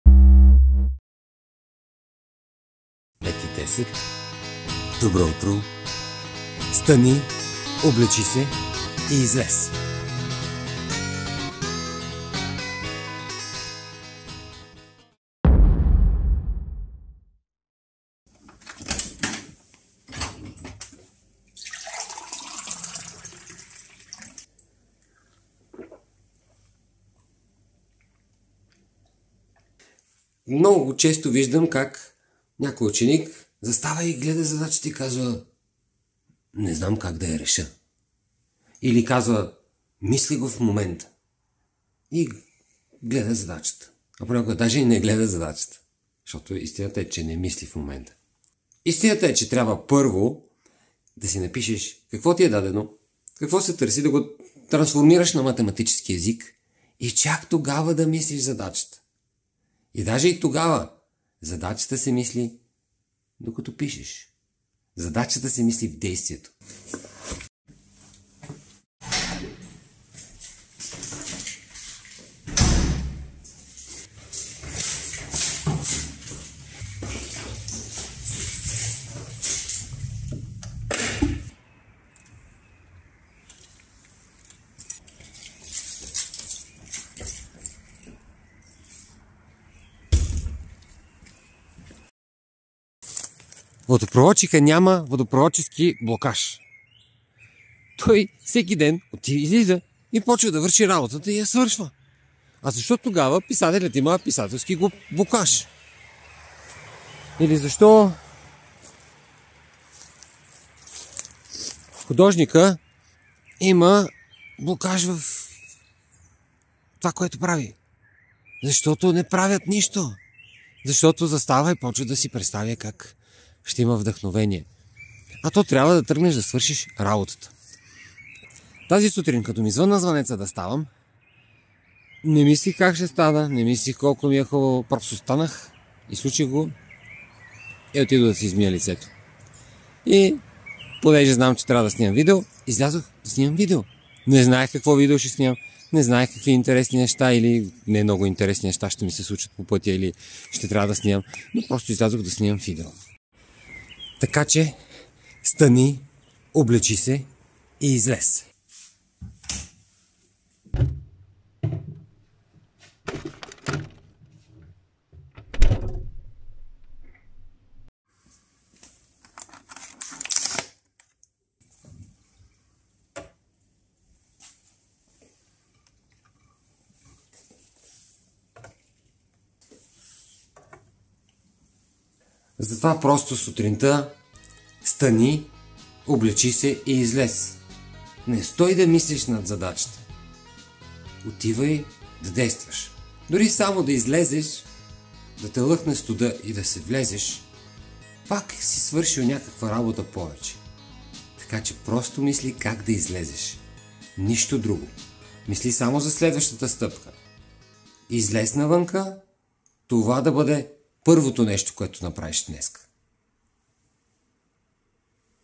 Стани, облечи се и излез
Реших да си направя насърчаващо видео, понеже ме наляга мързелът от време на време.

Много често виждам ученици да седят над задачата и да я умуват, вместо да се запретнат и да започнат да я решават. Същото се случва и с началото на деня, когато като чуем алармата, вместо да станем, ние продължаваме да се въргаляме под завивките. Затова стани, облечи се и излез – нека това да бъде първото нещо, което напарвиш в деня си.